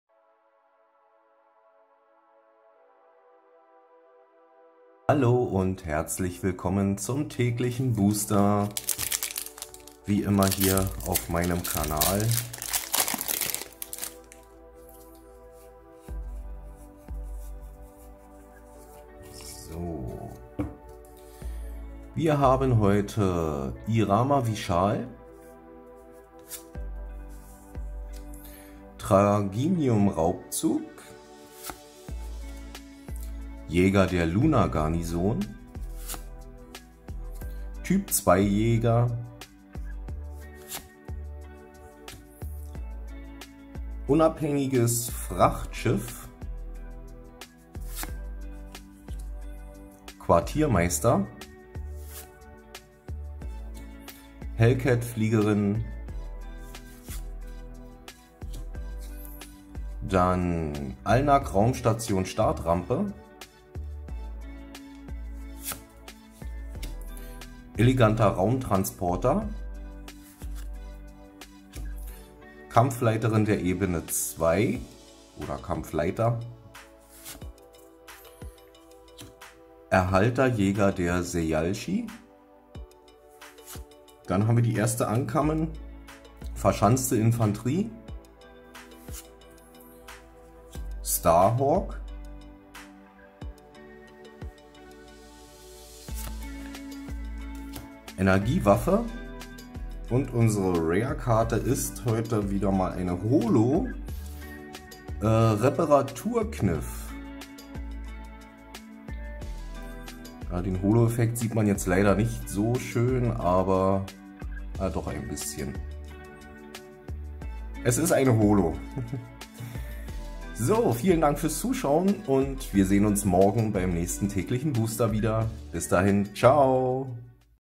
Der tägliche Booster 30 - WARS - Die Ankunft
Beim WARS Sammelkartenspiel kämpfen zwei Spieler um die Herrschaft in einer fantasievollen Zukunftswelt. Deine Karten stellen Personen, Raumschiffe und andere Ressourcen, unter deinem Befehl dar. Du wirst sie einsetzen, um deinen Gegner seine Energiequellen zu entziehen und um sie in den Kampf gegen gegnerische Streitkräfte zu schicken.
Das Set umfasst 330 Karten. 15 Karten pro Booster und 36 Booster
im Display.
Erschienen im Jahr 2004.